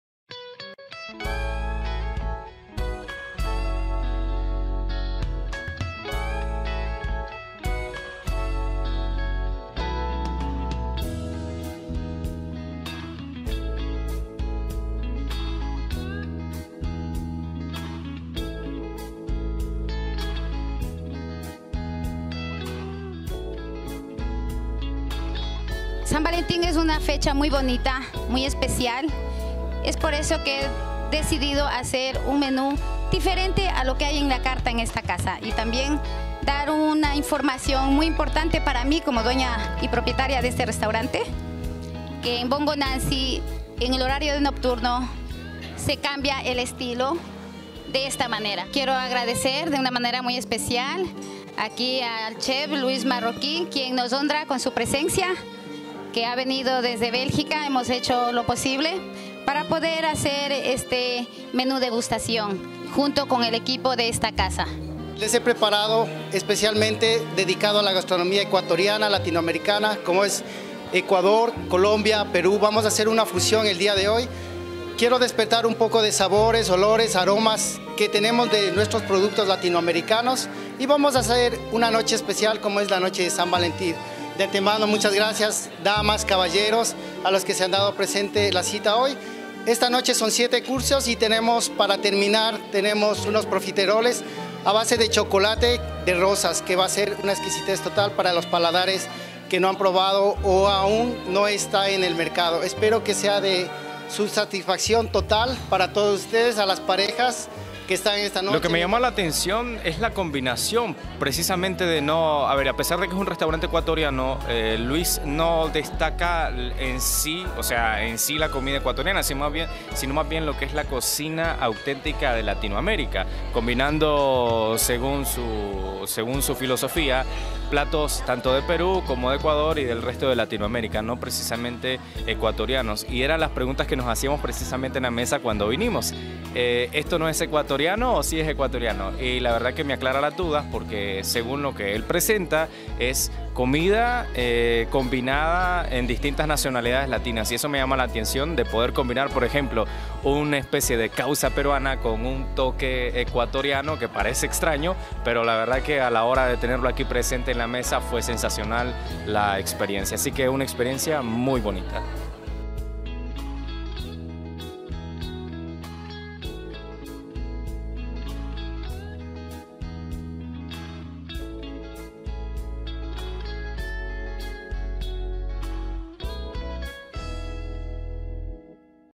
San Valentín Cenas Románticas BongoNancyRestaurante Canal Evento
El chef ecuatoriano LuisCastilloMarroquí que acudió desde su residencia en Bélgica para darlo todo en Madrid para que los comensales del "Restaurante Bongo Nancy" sean sorprendidos.
➡️Invitados especiales y música  a ritmo de violín hicieron una noche elegante y una estancia acorde a un ambiente de San Valentín.
🌺 Felicitaciones por ese gran trabajo en equipo del personal de cocina y en barra. Queda inaugurada la nueva temporada de CENAS Y CÓCTELES 🍹en
La comunicadora y corresponsal de LaNuevaTvEcuador y Canal RTU Patricia Morales, presentó este evento gastronómico.
BONGO NANCY, ¡ EL SABOR DE LA BUENA COCINA LATINA! __ Invitados a ser parte con este video resumen.👌 Y ya saben BongoNancy os espera. 💘
Calle de Parvillas Altas 27 Madrid